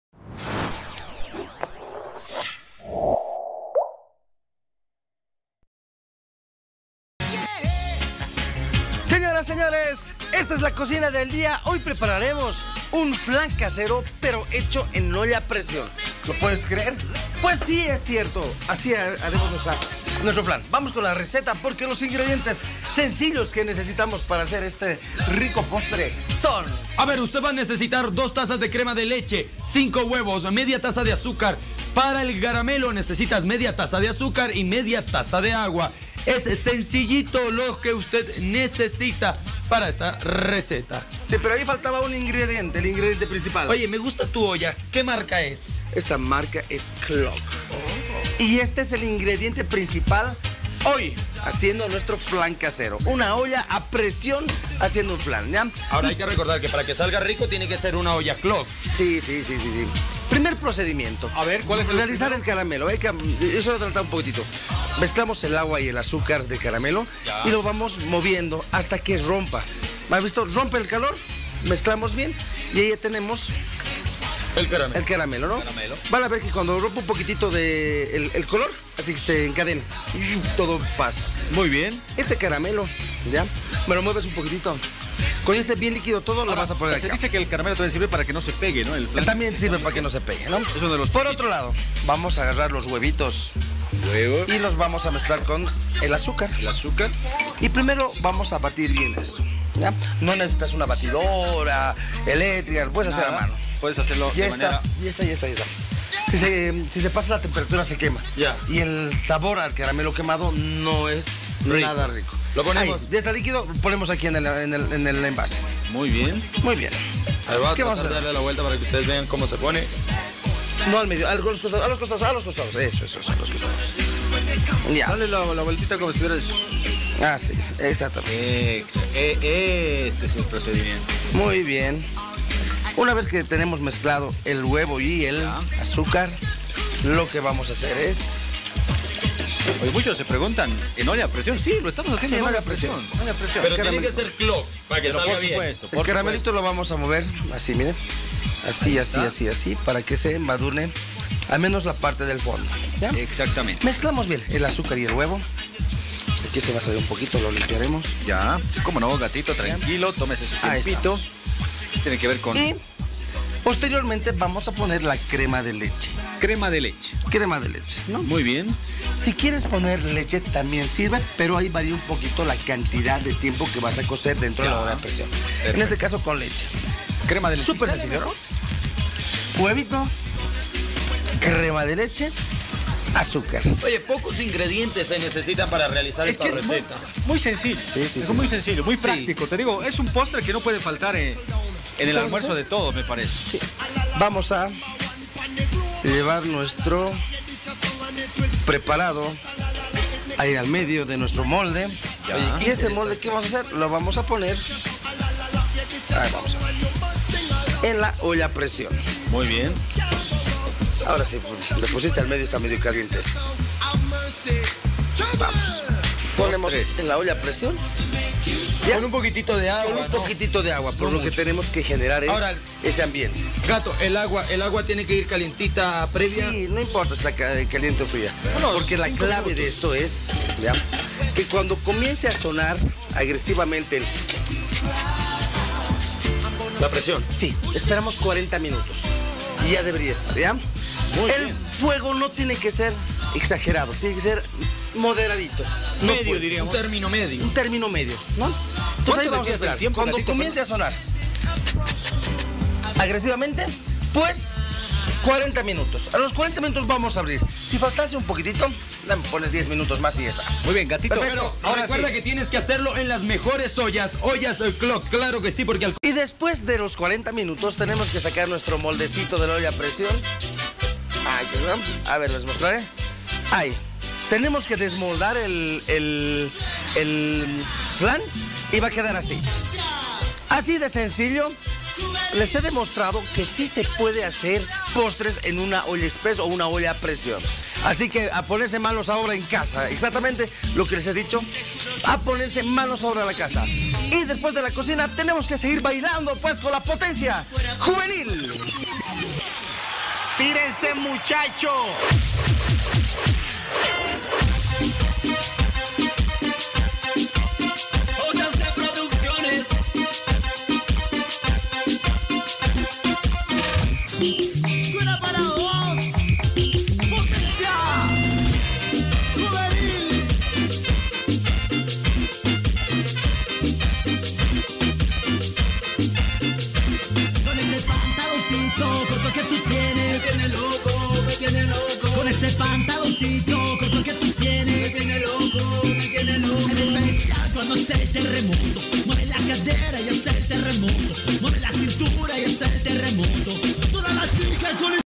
Preparamos una deliciosa receta de ''Flan casero en olla de presión''
En el segmento de la ''Cocina al Día'' le mostramos una deliciosa receta que le permitirá preparar un sabroso ''Flan casero en olla de presión'' desde la comodidad de su propio hogar con mucha facilidad.